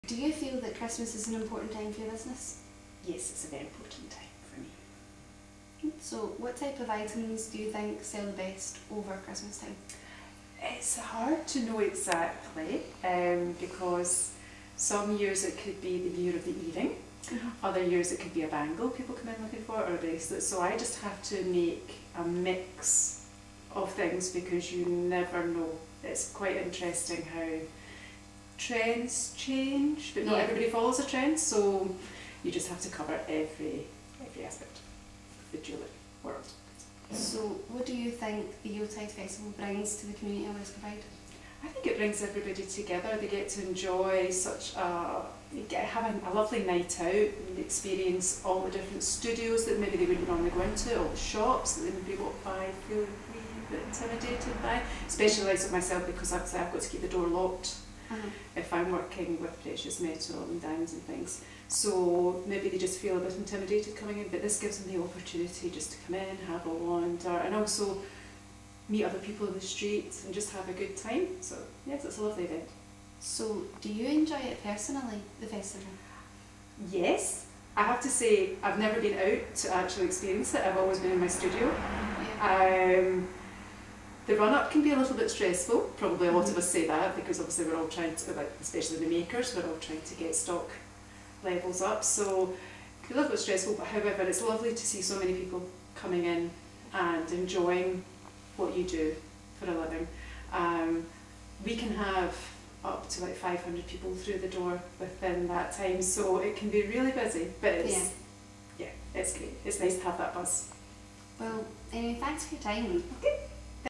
Yuletide Mentoring Project: Marion E. Kane Interview
Kelsey McNee of Ardrossan Academy interviews Silversmith Marion E. Kane about all things Christmas and West Kilbride Yuletide.